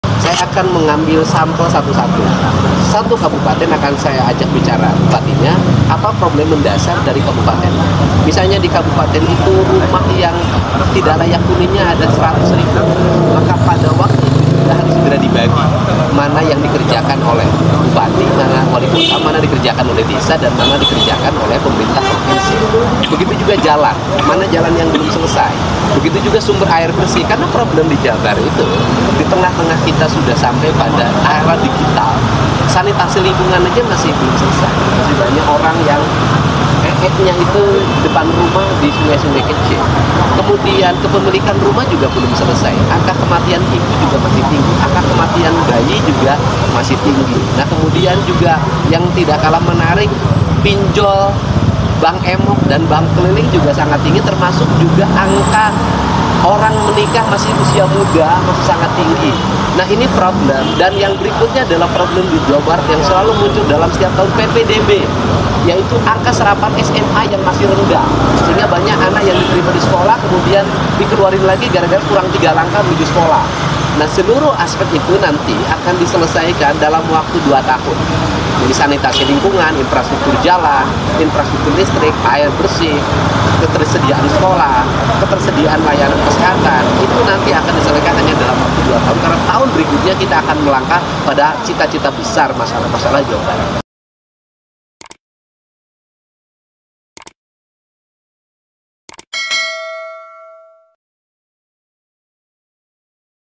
Dedi Mulyadi Janji Selesaikan Masalah Jabar dalam 2 Tahun, dari Sanitasi hingga Infrastruktur
Dedi Mulyadi berjanji untuk menyelesaikan pelbagai masalah yang ada di masyarakat jika dirinya terpilih menjadi gubernur Jawa Barat.

Hal itu disampaikan Dedi usai menerima formulir B1-KWK dari DPP Partai Solidaritas Indonesia (PSI), Senin (26/8/2024).

Nah seluruh aspek (permasalahan) itu akan diselesaikan dalam kurun waktu dua tahun. Mulai dari (permasalahan) sanitasi lingkungan, infrastruktur jalan, infrastruktur listrik, air bersih, hingga ketersediaan layanan kesehatan, ujar dia kepada wartawan.

Walau begitu, kata Dedi, dirinya tak bisa bekerja seorang diri.

Ia nantinya akan berkolaborasi dengan pemerintah desa hingga pemerintah kota atau kabupaten.

Saya akan mengambil sampel satu-satu. Satu kabupaten contohnya, akan saya ajak bicara bupatinya, apa problem mendasar dari wilayah tersebut. Misalnya di kabupaten itu rumah yang tidak layak huninya ada 100.000. Maka pada waktu itu harus segera dibagi, mana yang dikerjakan oleh bupati, mana yang dikerjakan desa, mana yang dikerjakan oleh pemerintah provinsi, tegas dia.

Penulis Naskah: Dzaky Nurcahyo
Video Jurnalis: Dzaky Nurcahyo
Video Editor: Dzaky Nurcahyo
Produser: Adil Pradipta Huwa